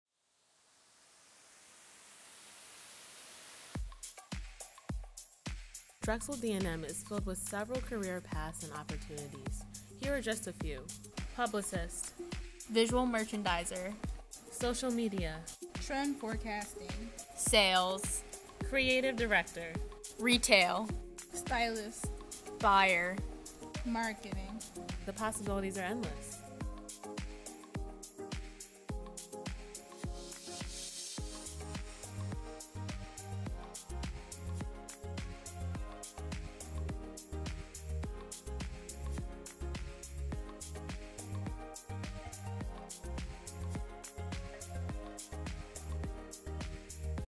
Drexel University: Design and Merchandising
A Design & Merchandising degree combines creativity and business across the fashion and lifestyle goods industries. Students in the Design & Merchandising Program in the Westphal College of Media Arts & Design develop an appreciation for style and product quality, learn to communicate verbally and visually through traditional and emerging media, and gain the business knowledge and skills to promote goods and services in the global marketplace.
Drexel’s Design & Merchandising Program is ranked 10th in the United States and 2nd among East Coast schools. (Fashion-Schools.org) Students gain the experience and leadership skills to make merchandising and marketing decisions in the classroom and through Drexel's renowned cooperative work experience. While working locally, nationally and/or internationally, they gain valuable industry experience and develop a worldwide consciousness and a professional network, as well as business savvy.